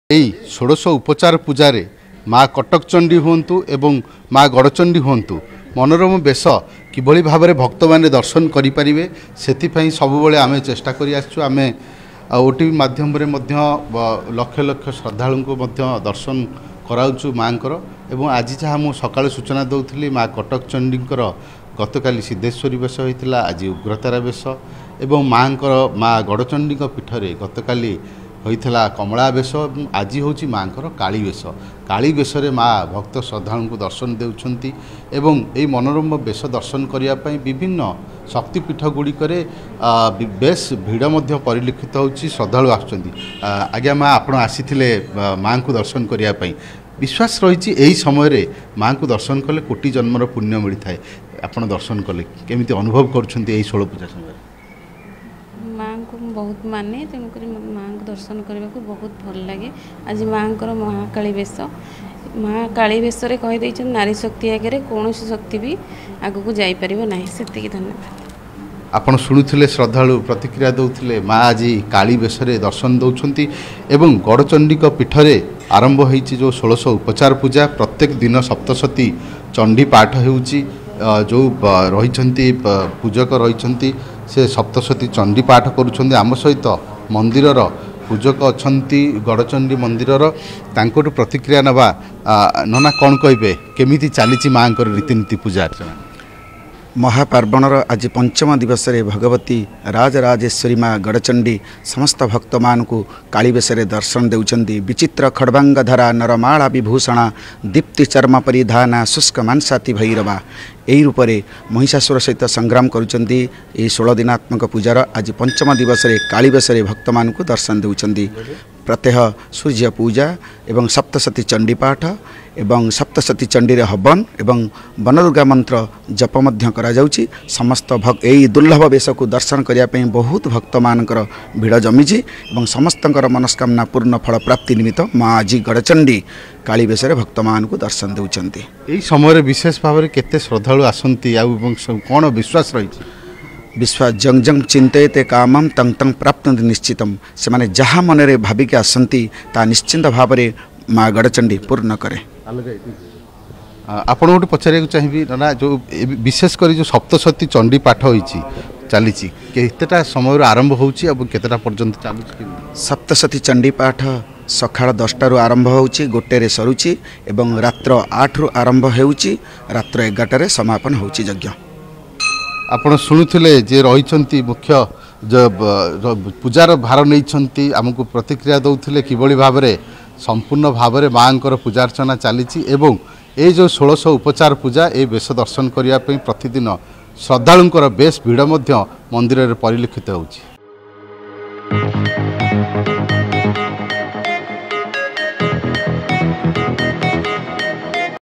ନର ମାଳା ପିନ୍ଧି ଦର୍ଶନ ଦେଉଛନ୍ତି ମା’ଚଣ୍ଡୀ | Odisha Reporter
ମା'ଗଡ଼ଚଣ୍ଡୀ ଆଜି ଦେବୀ ଉଗ୍ରତାରା ବେଶରେ ଦର୍ଶନ ଦେଉଛନ୍ତି । ଶାରଦୀୟ ଦୁର୍ଗାପୂଜା ଲାଗି କଟକର ବିଭିନ୍ନ ଶକ୍ତିପୀଠ ଗୁଡ଼ିକରେ ଆରମ୍ଭ ହୋଇଛି ଶରତ କାଳୀନ ଷୋହଳ ଦିନାତ୍ମକ ପୂଜା ଓ ବେଶ । ଆଜି ପଞ୍ଚମ ଦିନରେ ଉଗ୍ରତାରା ରୂପ ଧାରଣ କରିଛନ୍ତି କଟକର ମା'ଗଡ଼ଚଣ୍ଡୀ।